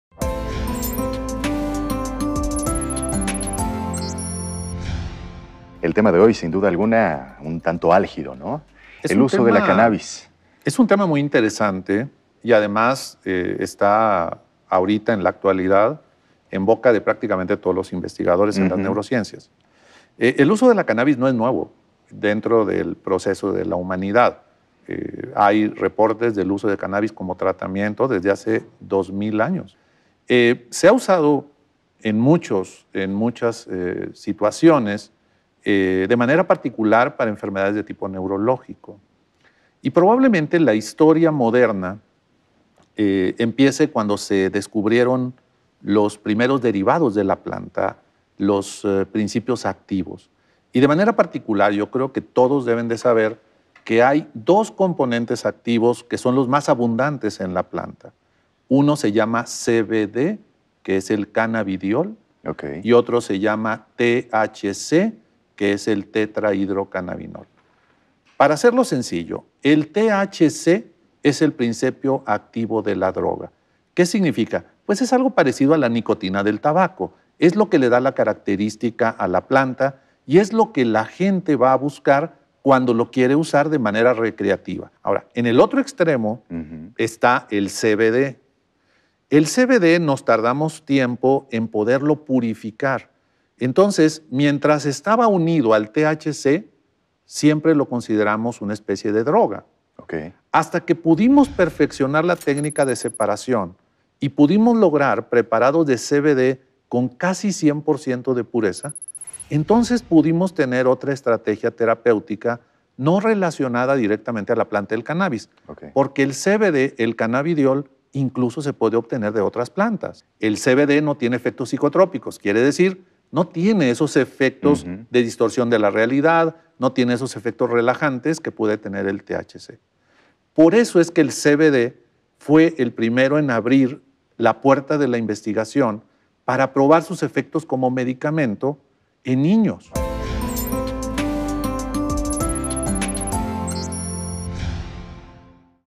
Uso médico del cannabis
El jefe del departamento de neurociencias del Instituto Nacional de Perinatología Saúl Garza Morales nos explica cómo la ciencia ha conseguido limpiar el cannabis de su componente alucinógeno para usarlo en la cura de enfermedades de tipo neurológico.